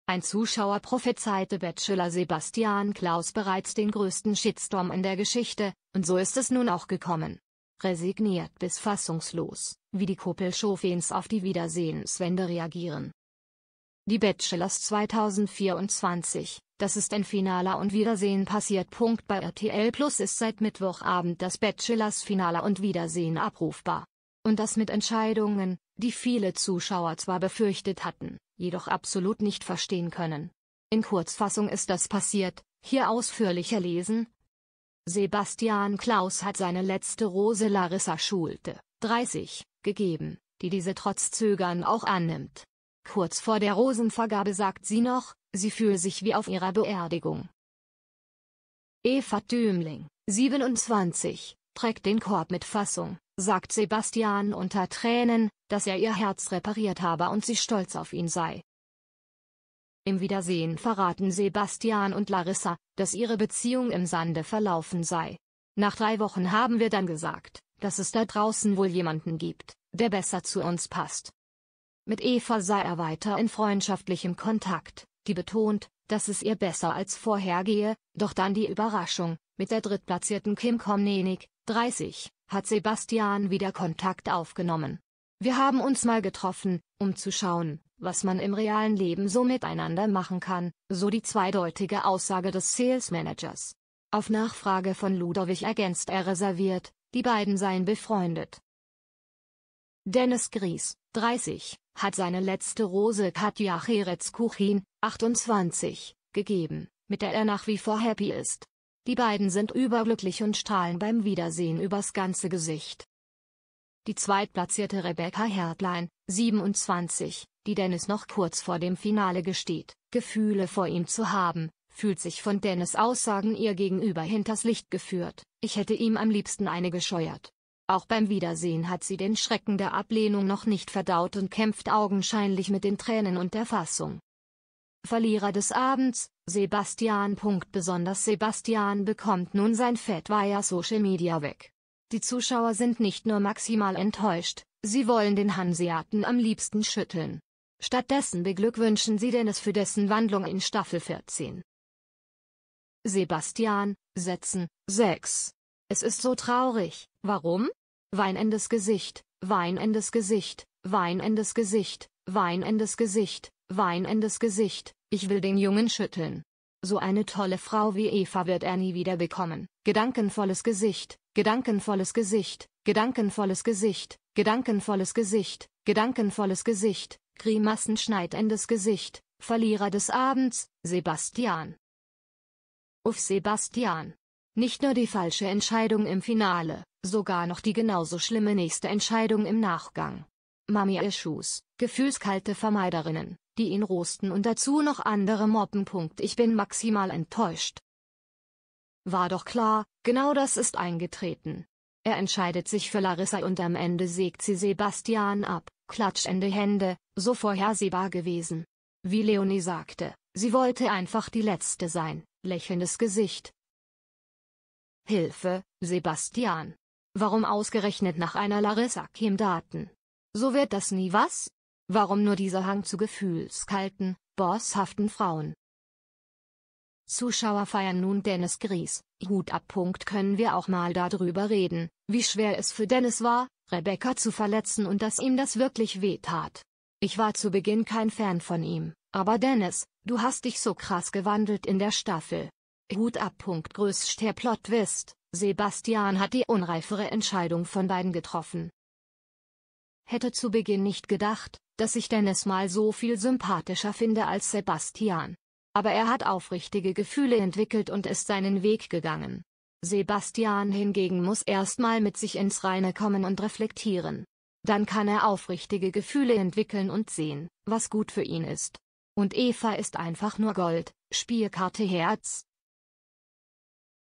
Bachelors Wiedersehens Wut Sebastian, Setzen 6!
Ein Zuschauer prophezeite "Bachelor" Sebastian Klaus bereits den "größten Shitstorm in der Geschichte" - und so ist es nun auch gekommen.